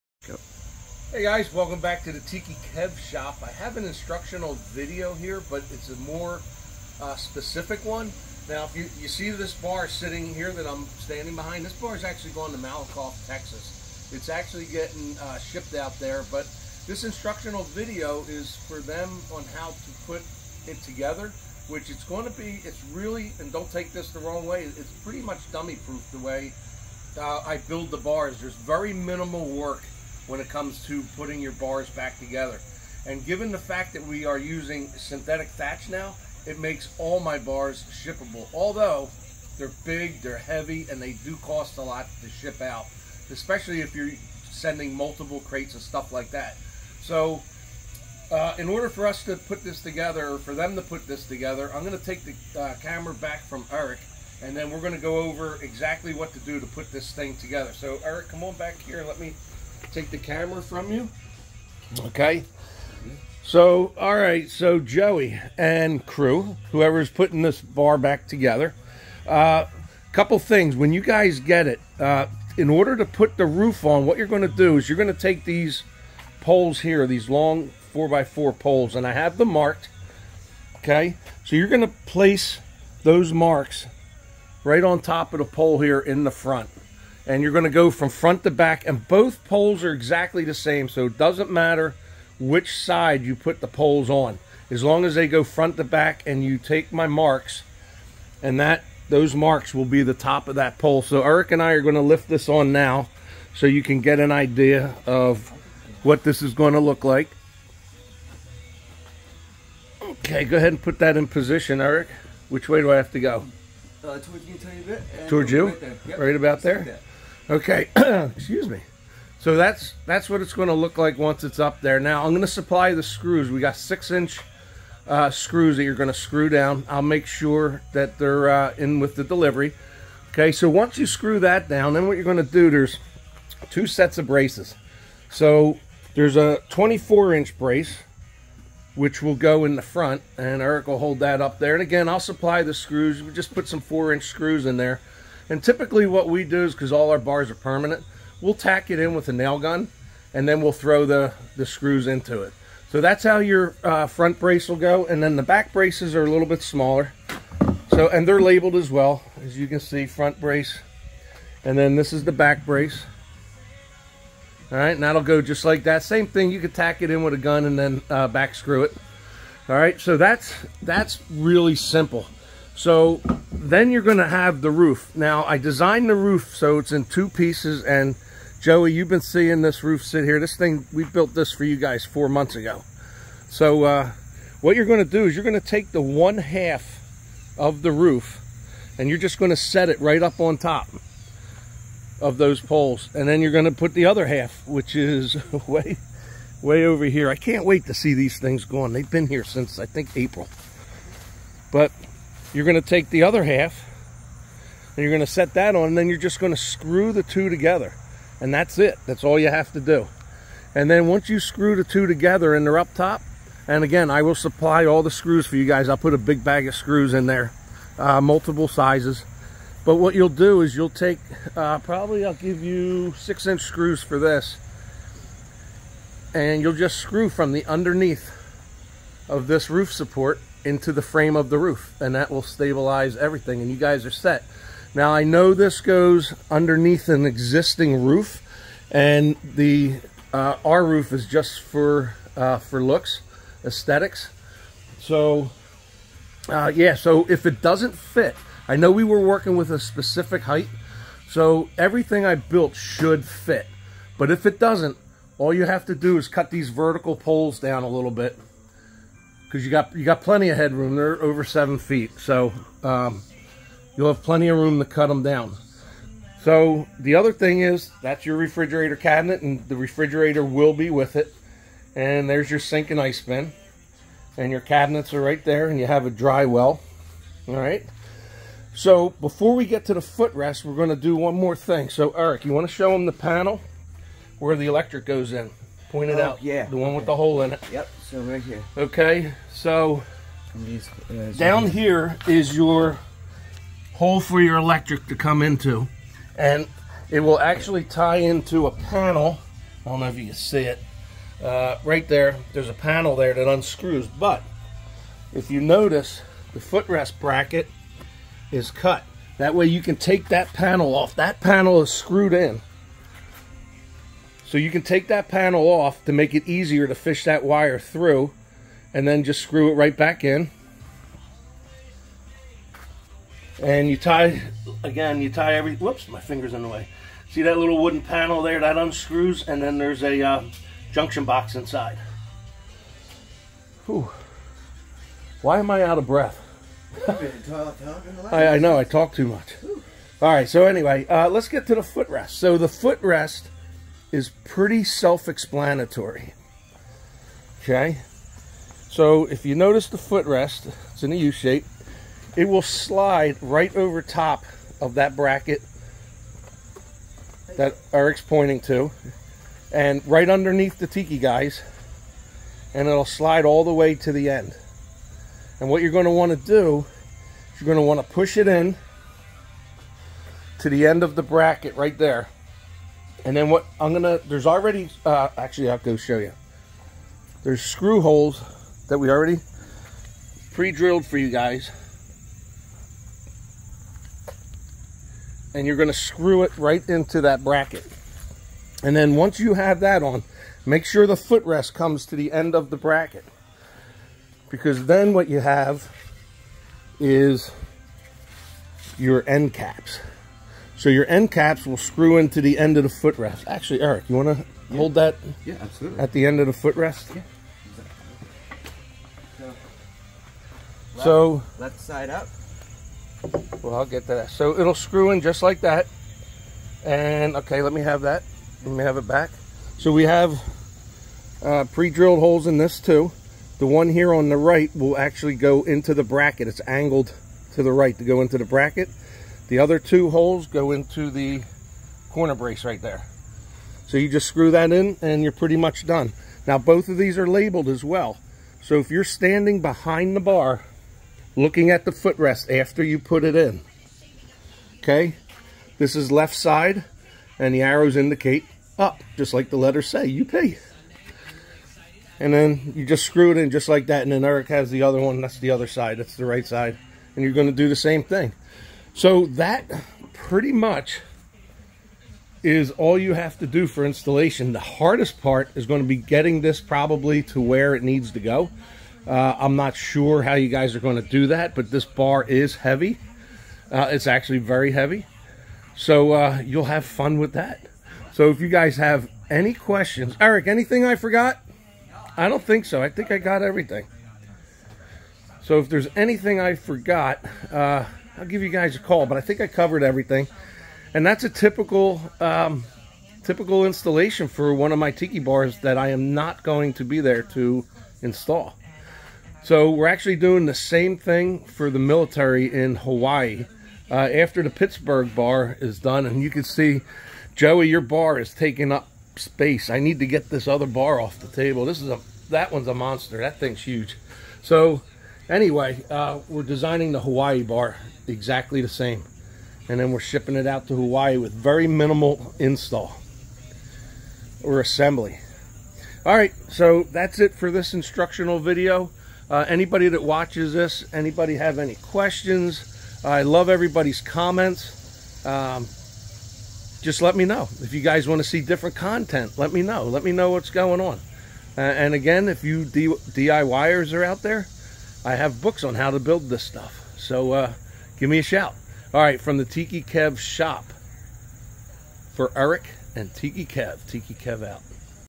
Tiki Kev's Malakoff Texas Tiki Bar instructional install video.
This is the instructional install video for the 10x7 Tiki Kev tiki bar I am shipping to Malakoff Texas with a 12x9 endureed viva 3.0 synthetic thatched roof for Bulldog Marine. comments and questions welcome.